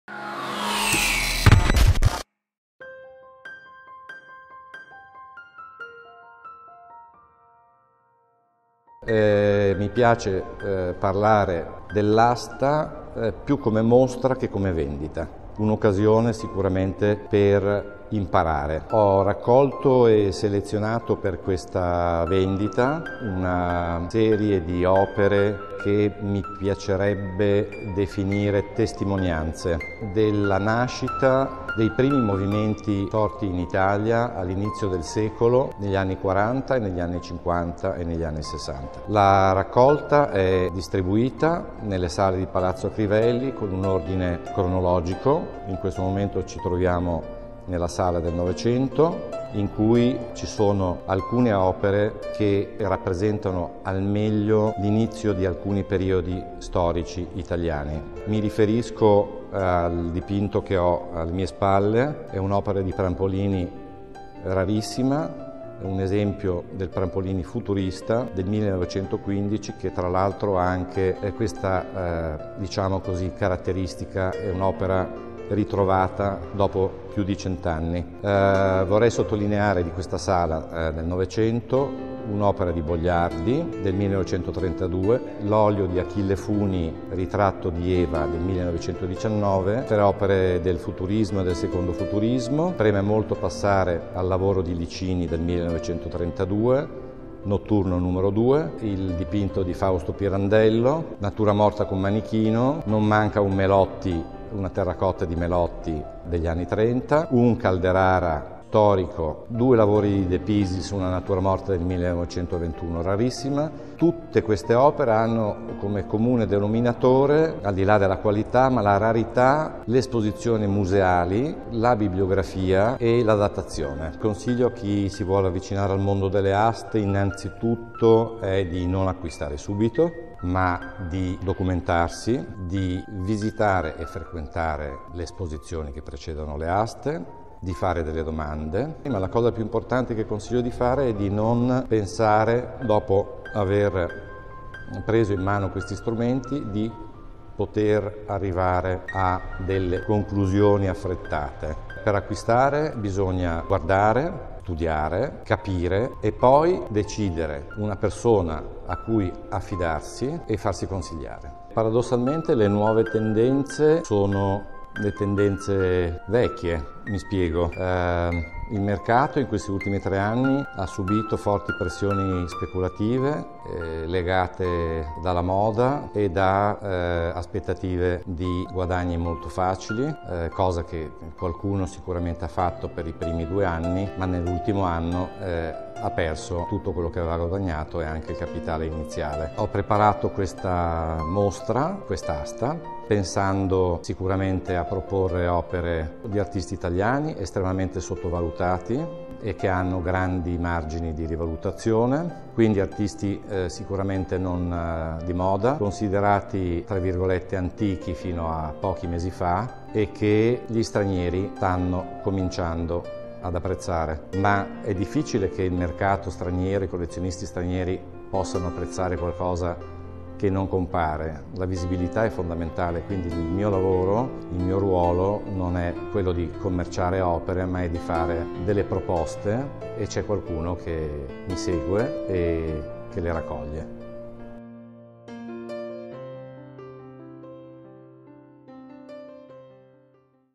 Freddy Battino ci racconta la prossima asta d'Arte Moderna e Contemporanea da Il Ponte a Milano
Il Direttore di Dipatimento di arte moderna e contemporanea Freddy Battino apre le porte di Palazzo Crivelli in Via Pontaccio 12 a Milano, dove ha sede la casa d'aste Il Ponte. In anteprima le opere della prossima asta del 12 giugno e alcuni suggerimenti dall'esperto.

Casa d'aste Il Ponte - Milano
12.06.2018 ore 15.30